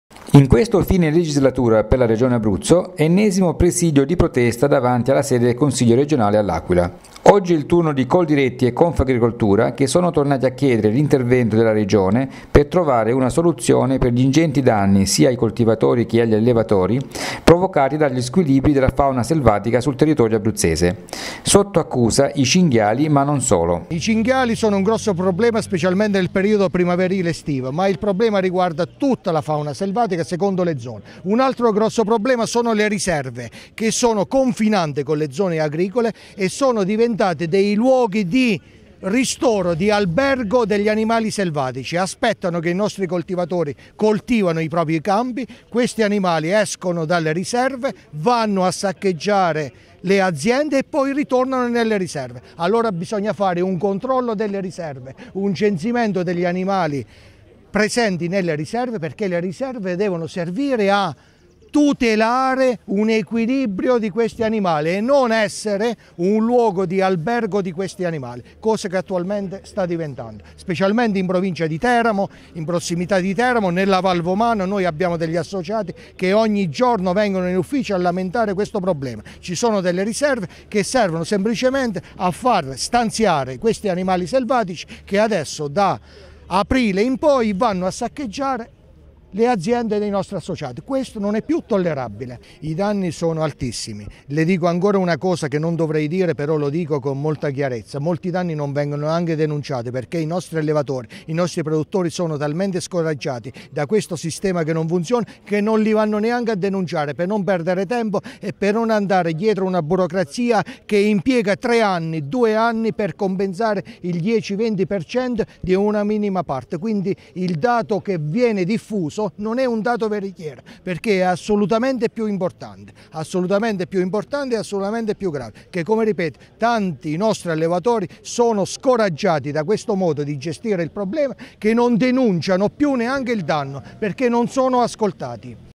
Danni fauna selvatica: la protesta di agricoltori e allevatori
In questo fine legislatura per la Regione Abruzzo, ennesimo presidio di protesta da davanti alla sede del Consiglio Regionale, a L'Aquila. Coldiretti e Confagricoltura sono tornati a chiedere l'intervento della Regione per trovare una soluzione per gli ingenti danni sia ai coltivatori che agli allevatori provocato dallo squilibrio della fauna selvatica sul territorio abruzzese. Sotto accusa i cinghiali, ma non solo.